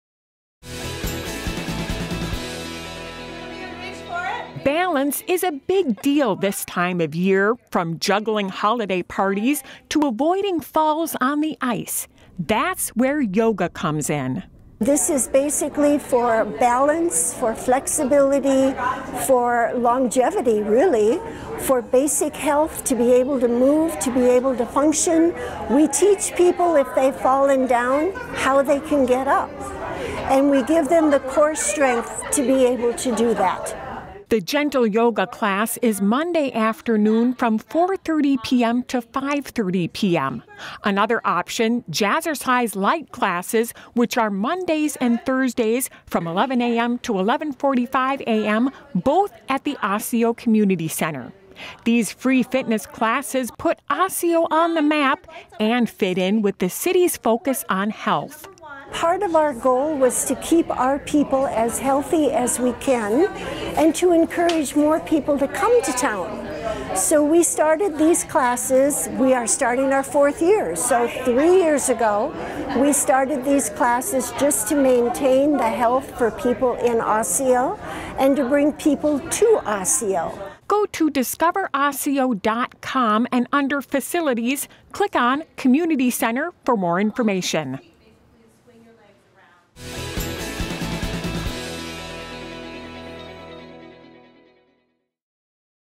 Keeping Fit In Osseo
Now is a great time to get a jump on your New Year's resolutions in Osseo.  The city offers free fitness classes at its community center. A Gentle Yoga class is Mondays from 4:30 PM - 5:30 PM and Jazzercise Lite classes are Mondays and Thursdays from 11:00 AM - 11:45 AM.